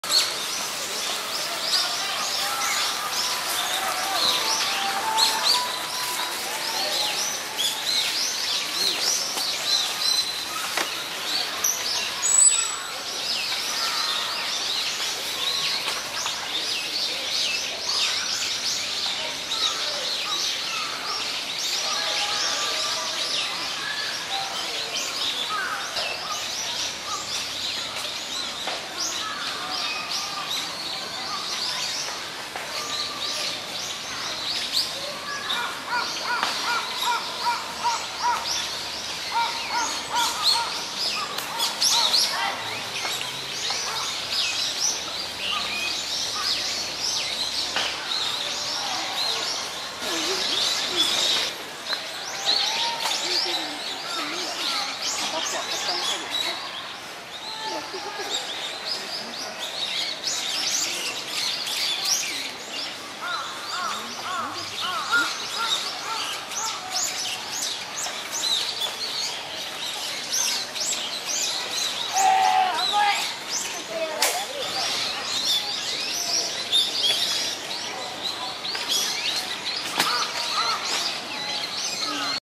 「 野鳥百景 」 ホシゴイ feather
秋色のプラタナスの中で寛ぐゴイサギの幼鳥